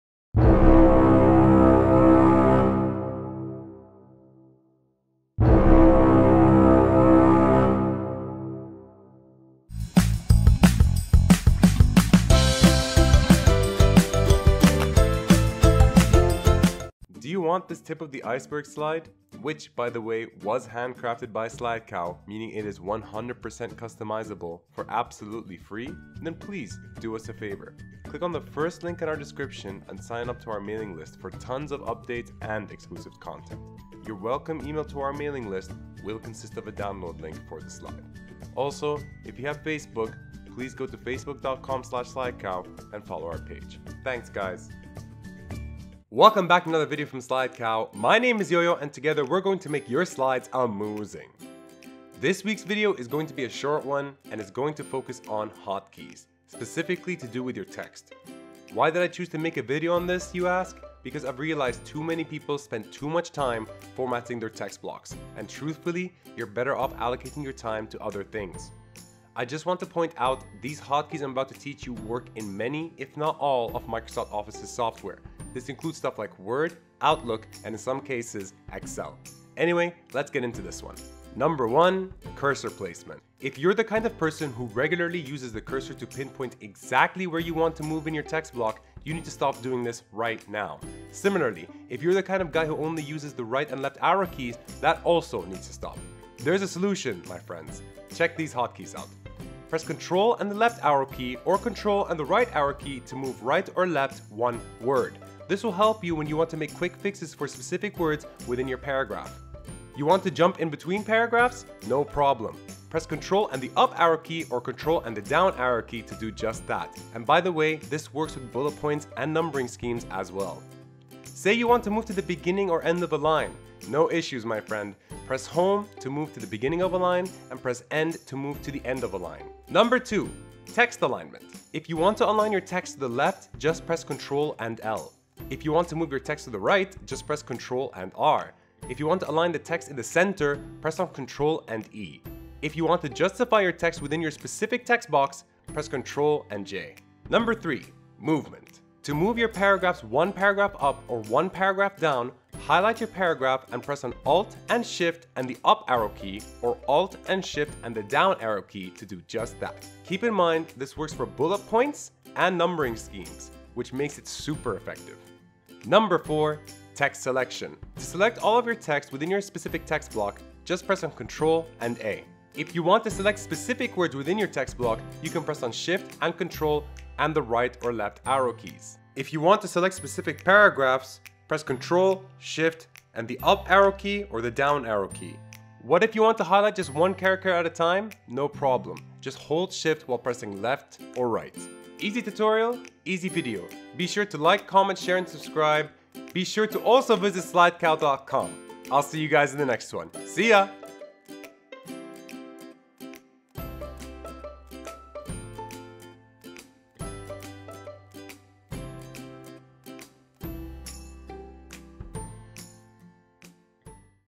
Try These 12 PowerPoint Text Editing Hotkeys to Help You Save Time!
We’ve done loads of videos on how to help you guys make sleek PowerPoint slides. This week’s tutorial is going to take on a different approach. It’s about saving time when drafting your slides. Specifically, we are going to teach you guys some text editing hotkeys.

**WHAT ARE TEXT EDITING HOT KEYS?**

No matter what PowerPoint slide we have, there is a 99.99% chance we are going to have some text in them. Problem is, I've seen people spend too much time formatting the text on their slides. Instead, they can dedicate some of that time to other areas. So I’ve combined a list of text editing hotkeys for us to make to make us of.

For your convenience, I’ve split them up into categories. I strongly recommend that you watch the video above for a live demonstration of all the hotkeys.

**CURSOR PLACEMENT**

- Press Control + Left / Right to move right or left one word.

- Press Control + Up / Down (this works with bullet points and numbering schemes as well).

- Press Home or Press End to move to the beginning/end of a line in a PowerPoint text block.

**TEXT ALIGNMENT**

- Press Control + L to align your text to the left of your PowerPoint text block.

- Press Control + R to align your text to the right of your PowerPoint text block.

- Press Control + E to align your text in the center of your PowerPoint text block.

- Press Control + J to justify your text within your PowerPoint text block.

**MOVEMENT**

After highlighting a paragraph, press Alt + Shift + Up / Down to move your paragraphs one paragraph up or one paragraph down.
Keep in mind this works for bullet points and numbering schemes as well which makes it super effective.

**TEXT SELECTION**

- Press Control + A to select all of your  text within your specific PowerPoint text block

- Press Shift + Control + Right / Left to select specific words within your PowerPoint text block.

- Press Shift + Control + Up / Down to select specific paragraphs within your PowerPoint text block.

- Press Shift + Right /  Left to select one character a time within your PowerPoint text block

Now, obviously, there are other text editing hotkeys that we can make use of. However, I think those are the main ones that will serve you well.

Be mindful that these hotkeys will work on most, if not all, of the Microsoft Office suite software. This includes stuff like Word, Outlook, even Excel in some cases.

Hope this helps you guys save time when making your awesome slides!